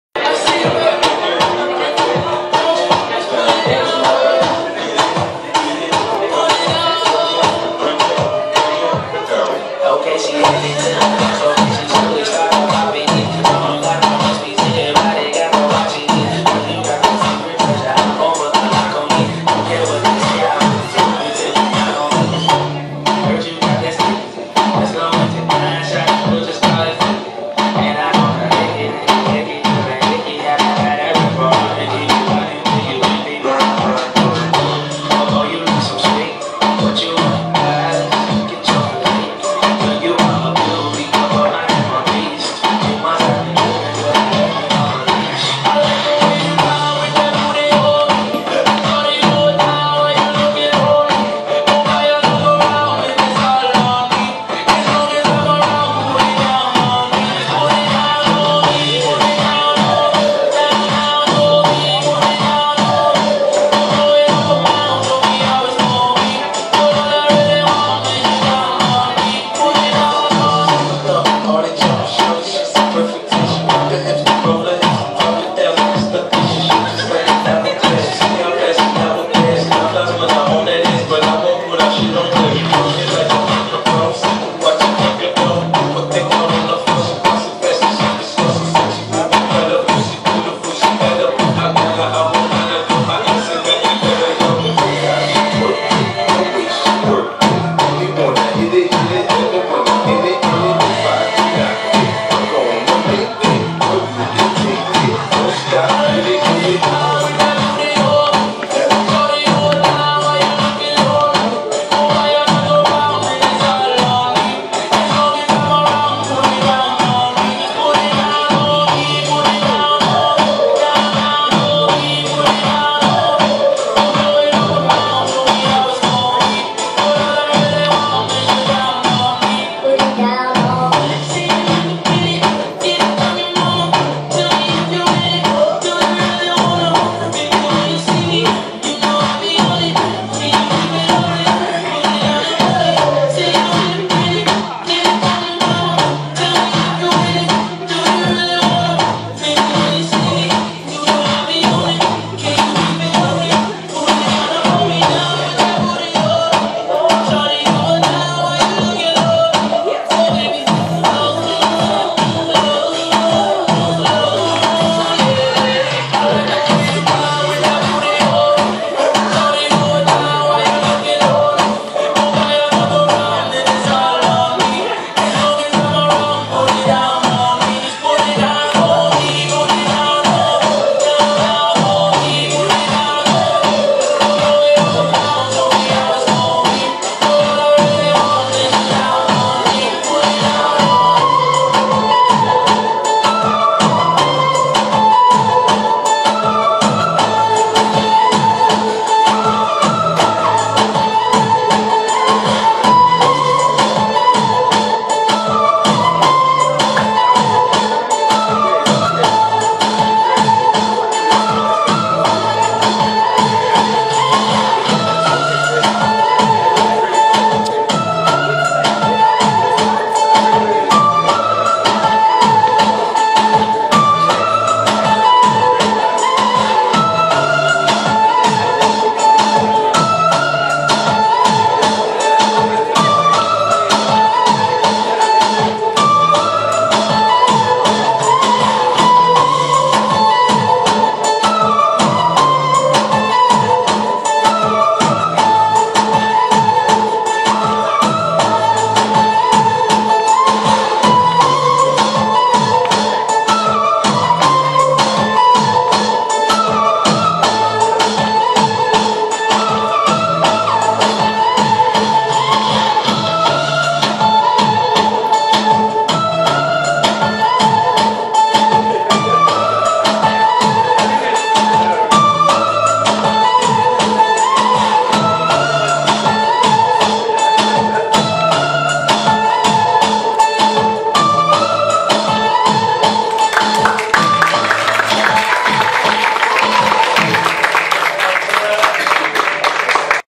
ANTHONY BREGA & LINDA BRAND
Zouk freestyle demo at 1st Royal Zouk & Kizomba festival 2012 in Gdynia, Poland